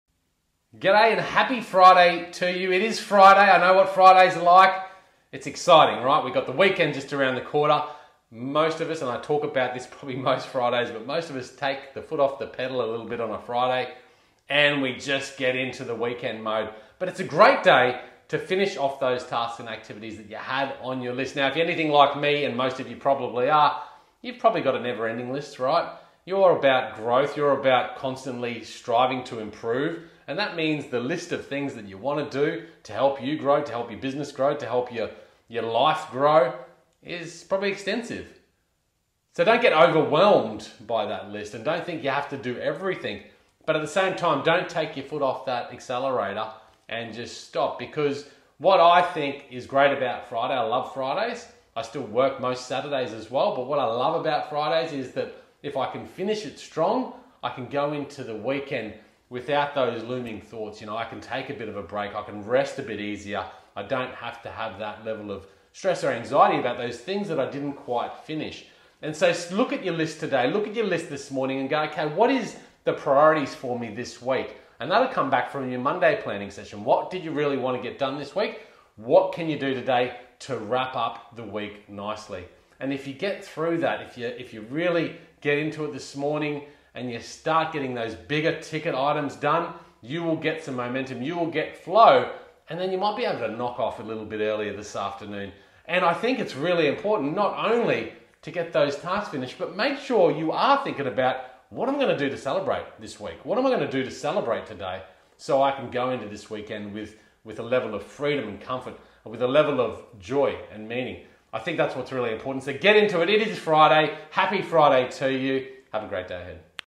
Motivational Daily - Let's Finish Out The Week Strong... HAPPY FRIDAY!!
🌟 Quick motivational talk for today! It's Friday, get into it! I love Fridays but don't take your foot off the accelerator. Keep pushing, look at your task list, and wrap up those big wins so you can enter the weekend with a sense of freedom and accomplishment. 🏆

Plan it out, celebrate the wins, and make sure to schedule some personal time too. Enjoy your Friday, celebrate your week, and make the most of it. 💥 💪